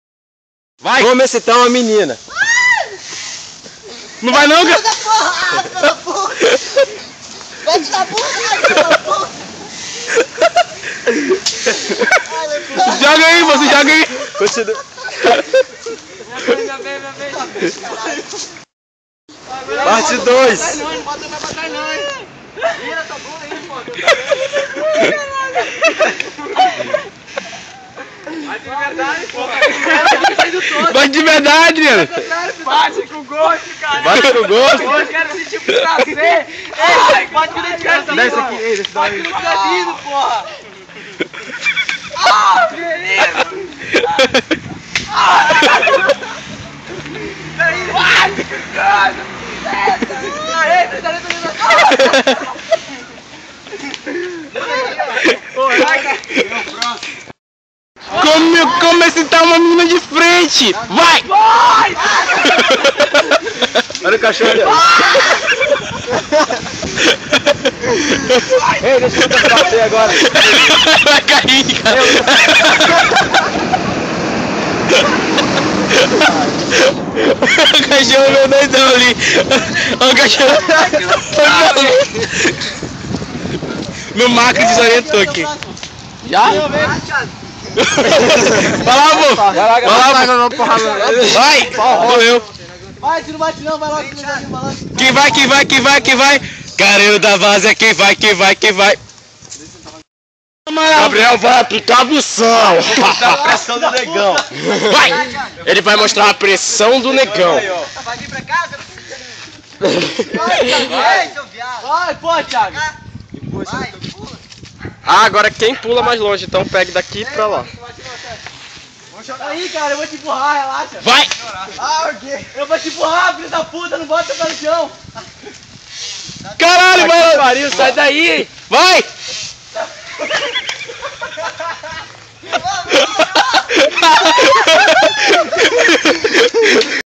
Como deixar uma garota feliz
Tutorial simples para deixar uma garota feliz.

Apresentando:

Adriano Gama (Menininha)
Caio Victor ROSA (Agressor 1 - fraco)
Gabriel Preto (Agressor 2 - Violento)
Thiago Câmara ( Figurante )
Lucas Silveira (Unútil)
Gabriel Santos (Câmera)

Vaii!